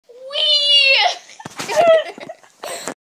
Weeeee
Me and my friend kitty doing a puppet show and this happened